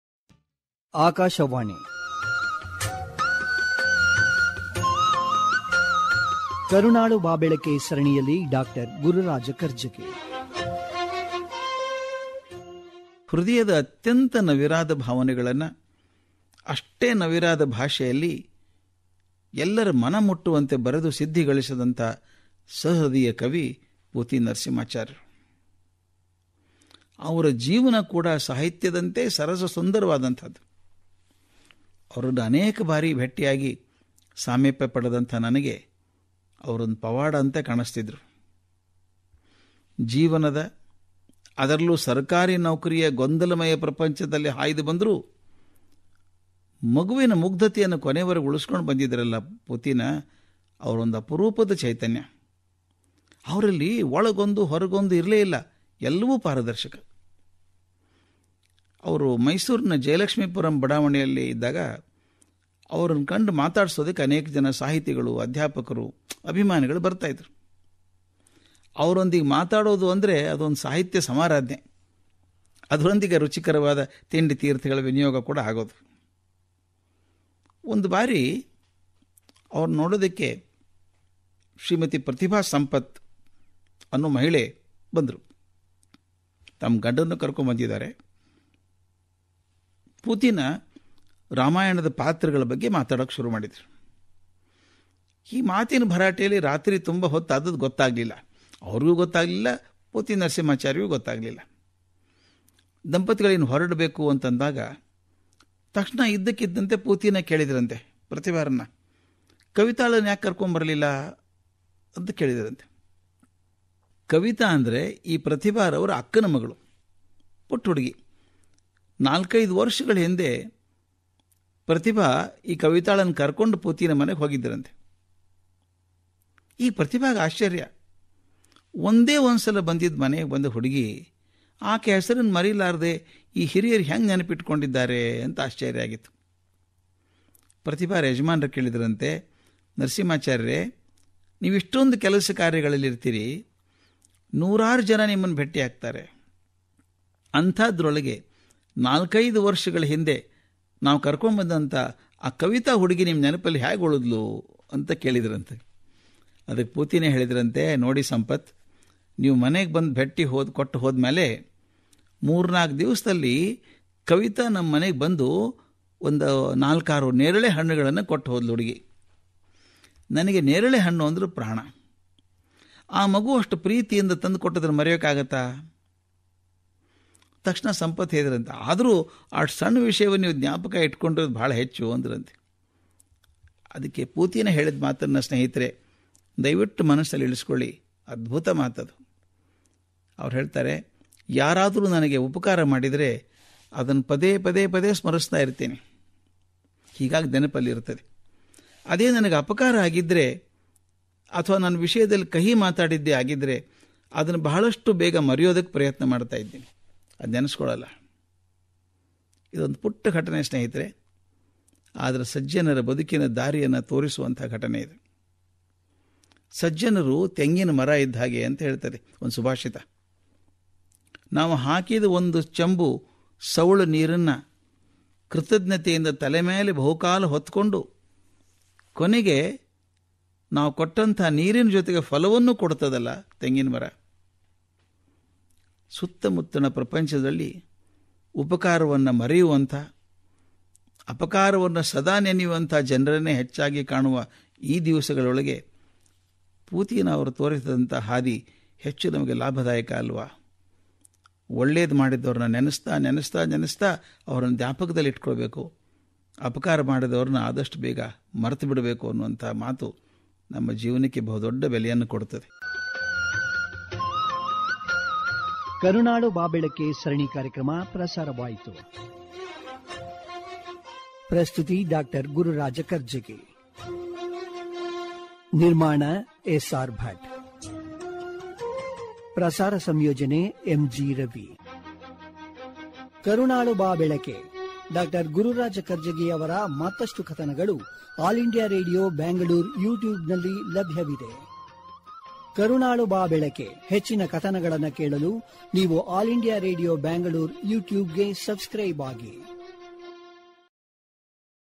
EP-347,Kaarunalu Baa Belake:-ಪು.ತಿ.ನ.ಅವರು ಉಪಕಾರ ಸ್ಮರಣೆ ಹೇಗಿರಬೇಕೆಂದು ತೋರಿಸಿದ ಘಟನೆ By DrGururajKarjagi
ಪು.ತಿ.ನ. ಉಪಕಾರಸ್ಮರಣೆಹೇಗಿರಬೇಕೆಂದುತೋರಿಸಿದಘಟನೆ.
Produced by S R Bhat (PROG Officer)